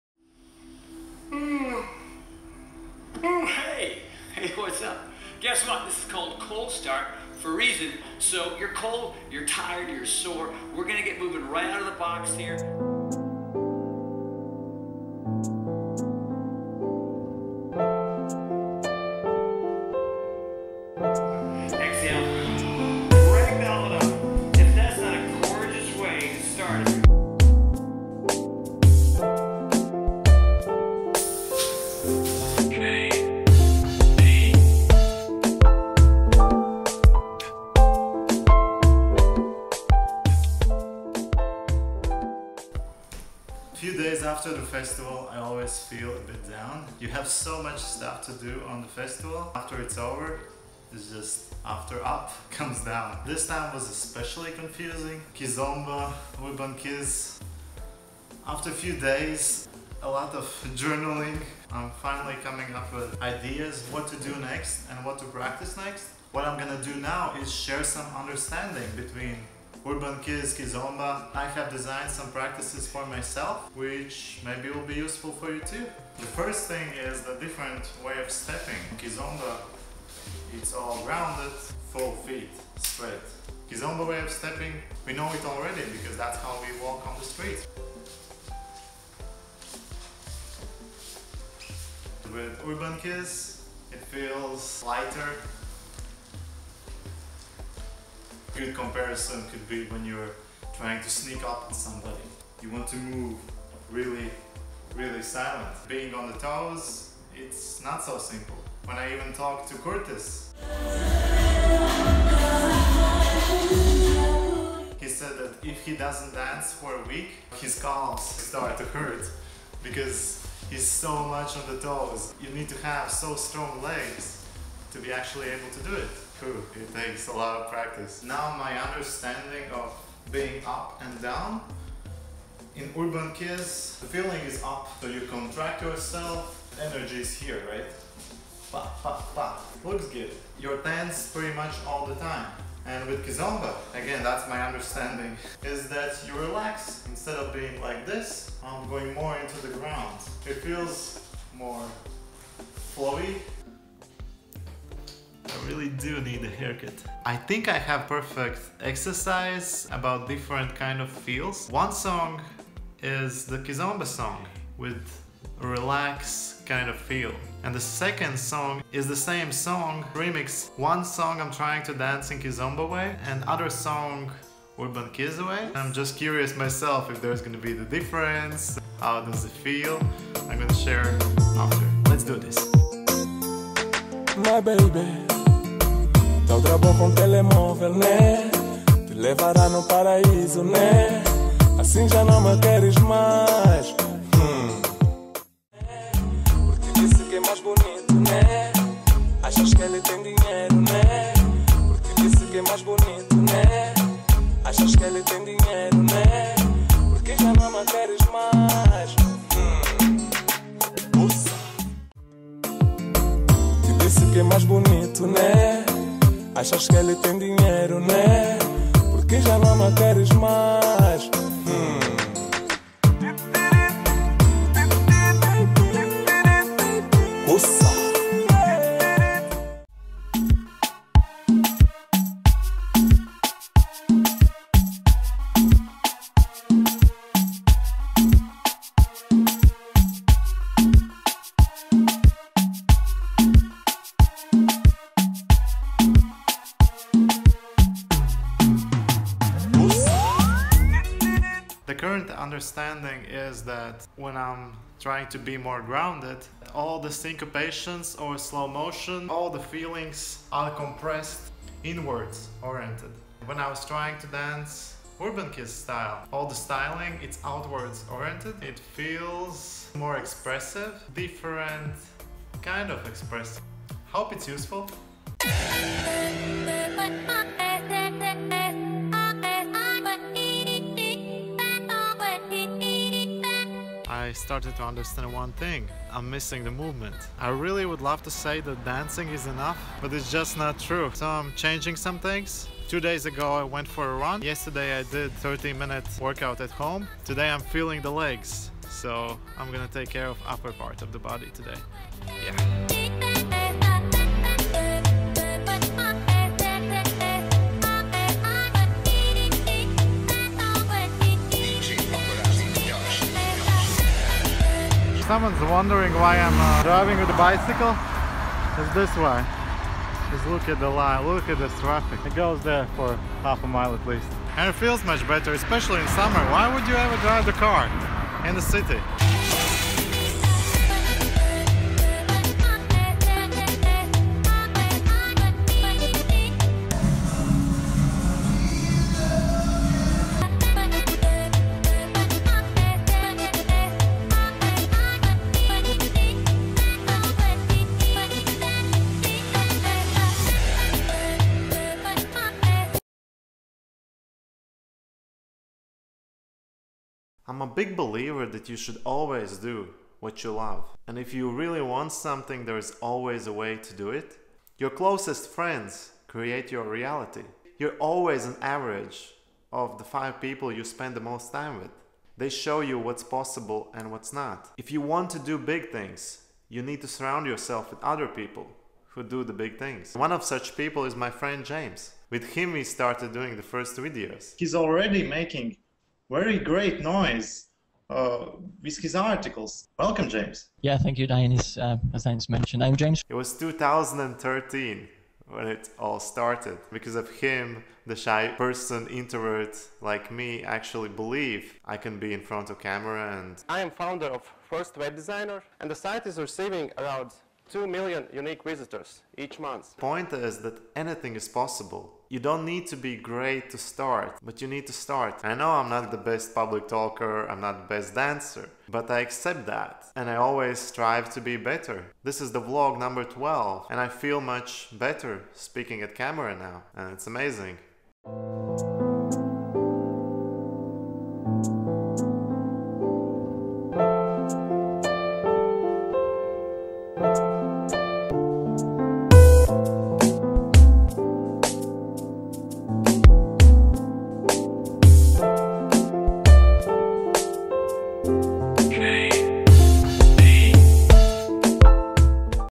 Differences Between Kizomba vs Urban Kiz
How does kizomba fare vs urbankiz? What are the main differences? Answers in the video!

Please note, that I am only student, not a teacher - and if you know better way of explaining things - please be a kind person and educate me and others!